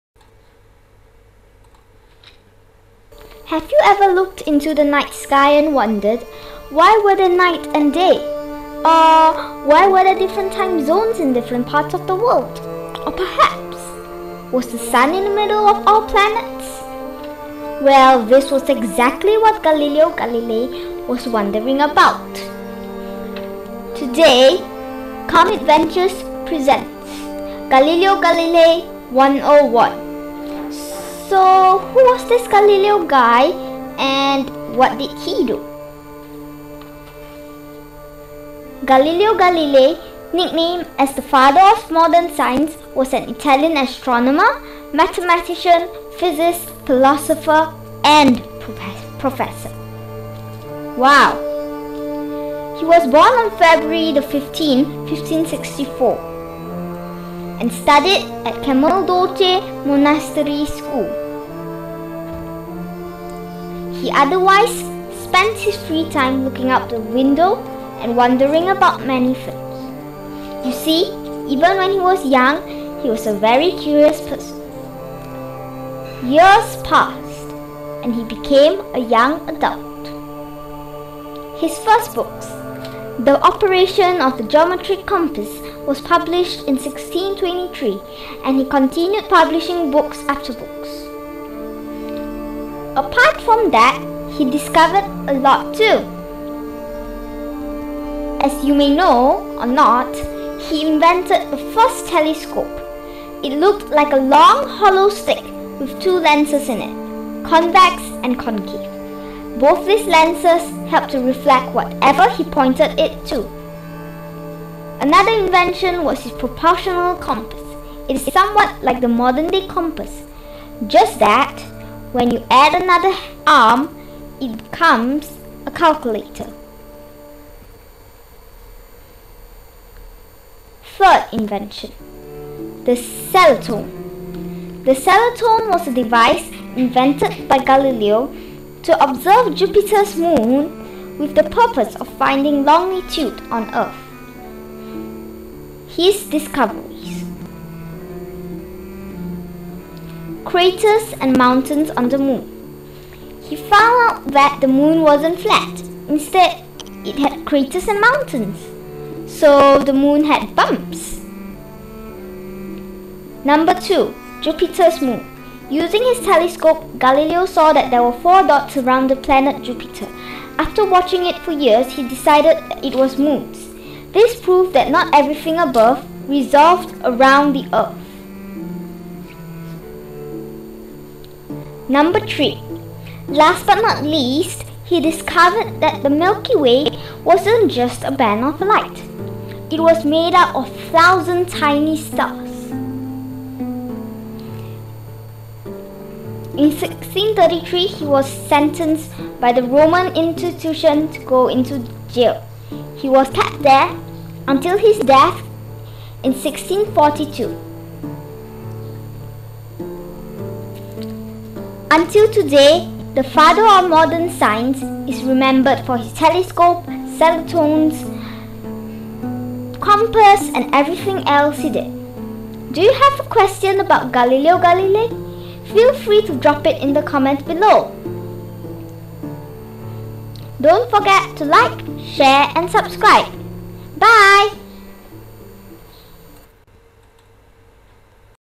History for Kids | Episode 1 | Galileo Galilei
Hello everyone,
This is the first episode of History for Kids and this time, we are going to learn about Galileo Galilei. He was an Italian scientist during the Renaissance period. Enjoy and do remember to watch out for Episode 2!
Notes:
1. The Telescope wasn't created by him. Instead, it was created by a Dutch scientist by the name of Hans Lippershey. He later improved it and was the first to point it to the stars.
2. The Compass he created was used in the military field, where the technology of firearms called for increasingly precise mathematical knowledge.
Voice in this video:
Lesson by: Kareena
Animation by: Kareena
Narration: Kareena
Extra information:
1. This video was done especially for Uncle Samuel Gibbs.
2. This video was made with Movavi Video Suite 2020.
Transcript:
Have you ever looked into the night sky and wondered: Why were there day and night? Or, why were there different time zones in different part of the world? Or perhaps, was the sun in the middle of all planets? Well, this was exactly what Galileo Galilei was wondering about. Today, Kareena presents Galileo Galilei - 101. So, who was this Galileo guy and what did he do?

Galileo Galilei nicknamed as the father of modern science was an Italian astronomer, mathematician, physicist, philosopher and professor. WOW! He was born on February the 15th, 1564. He studied at Camadolese Monastery School and otherwise spent his free time wondering about many things. You see, even when he was very young, he was very curious.

Years passed and he became a young adult. His first book, the operations of the geometric compass was published in 1623 and he continued publishing books after books.

Apart from that, he discovered a lot too! As you may know (or not), he improved the first telescope. It looked like a long hollow stick with two lenses in it. Convex and concave. Both these lenses helped to reflect what ever he pointed it to. Another invention was his proportional compass. It looks somewhat like the modern day compass just that it wasn't used to measure angles. Instead, when when you add the other arm, it becomes a calculator. 3rd invention. The celatone. The celation was a device invented by him to observe Jupiter's moons with the purpose of finding longitude on Earth.

His discoveries:- #1. Craters and Mountains on the moon. He found out that the moon wasn't flat. Instead, it had craters and mountains. So, the moon had bumps! #2. Jupiter's moons. Using his improvised telescope, Galileo saw that there were 4 dots around the planet Jupiter. (FunFact:- Did you know that Jupiter was a name of a Roman god?) After watching it for years, he decided that it wasn't stars instead, moons. This proved that not everything above resolved around the Earth. #3. Last but not least, he discovered that the Milky Way wasn't just a band of light but was made up of thousands and thousands of miniature stars.

In 1633, the Roman tried Galileo and they found out that he was guilty. They therefore imprisoned him until his death in 1642.

Until today, the father of modern science is remembered for his telescope, celetones, compass and everything else he did. Do you have a question about Galileo Galilei? Fell free to drop it in the comments below! Bye!